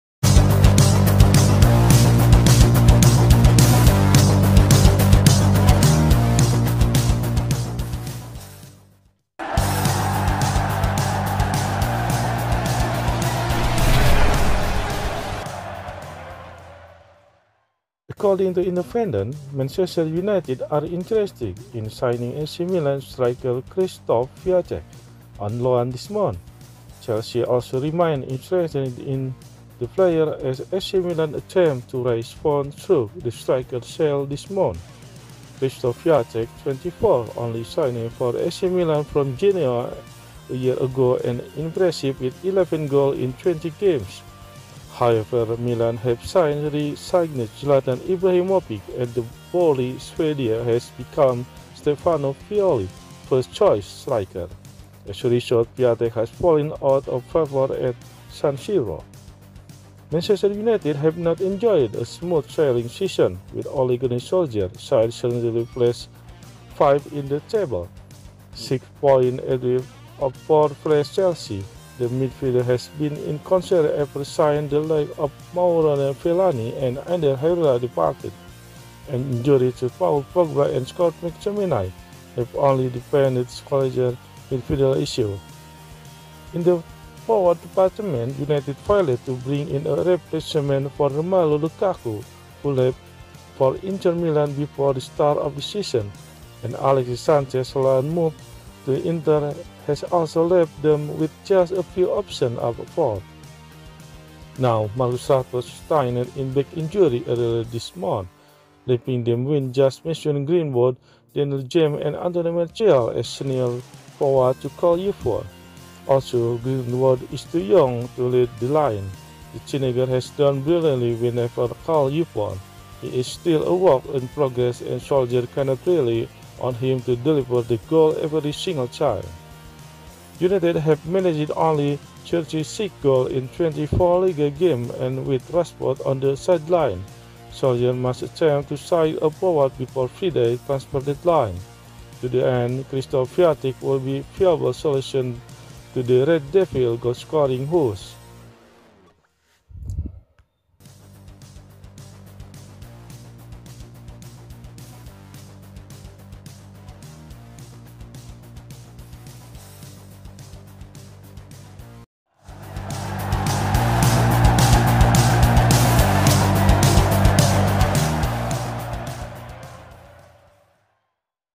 Man United signing this 24-year-old Serie A star would be a sensible move
According to The Independent, Manchester United are interested in signing AC Milan striker Krzysztof Piatek on loan this month. Chelsea also remain interested in the player as AC Milan attempt to raise funds through the striker’s sale this month.
Krzysztof Piatek, 24, only signed for AC Milan from Genoa a year ago and he impressed with 11 goals in 21 games. However, Milan have since re-signed Zlatan Ibrahimovic and the burly Swede has become Stefano Pioli’s first-choice striker. As a result, Piatek has fallen out of favour at San Siro